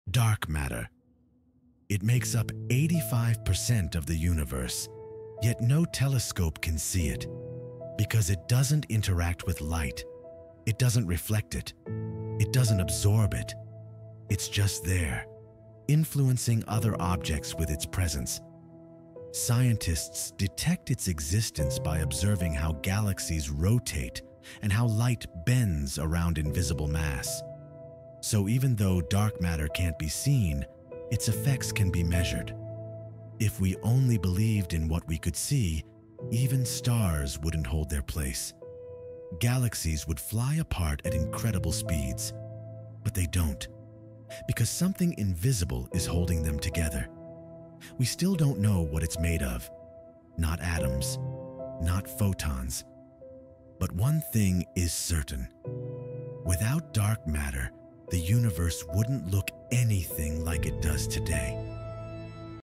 Dark Matter: Science’s Greatest Mystery!
What if everything we see — stars, planets, galaxies — was just a small part of reality?
Dark matter makes up 85% of the universe, yet we can’t see, touch, or detect it directly.
In this video, we explore the invisible force that shapes galaxies and holds the cosmos together.
The universe is stranger than we imagined.